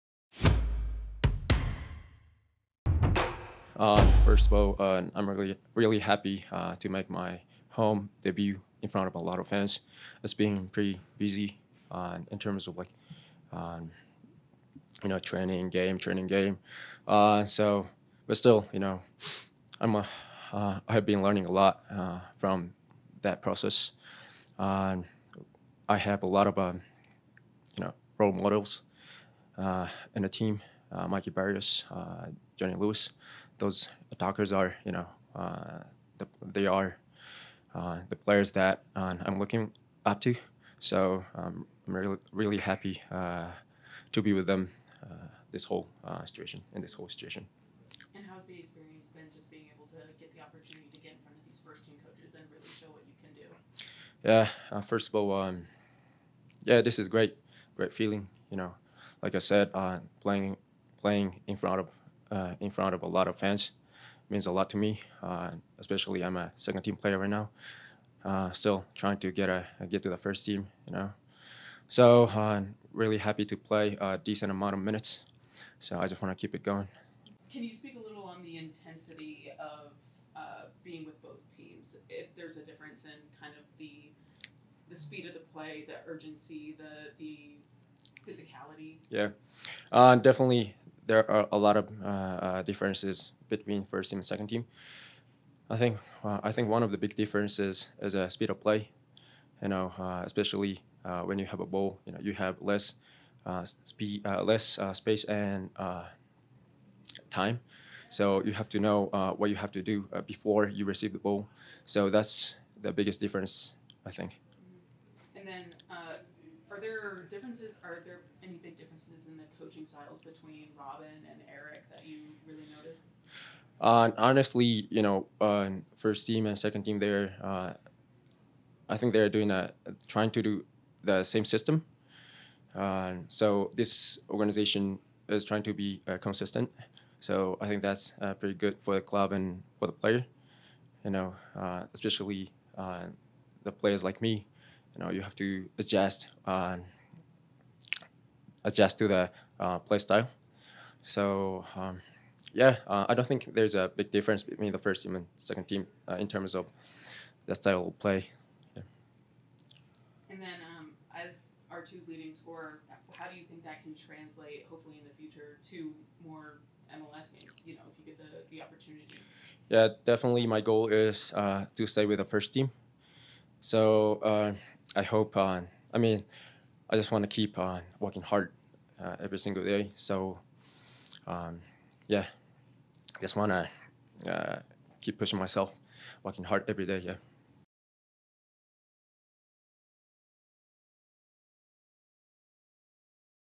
Yosuke Hanya reflects on gaining experience during call ups with the first team
Midfielder Yosuke Hanya discusses getting first-team reps during his call up from Rapids 2.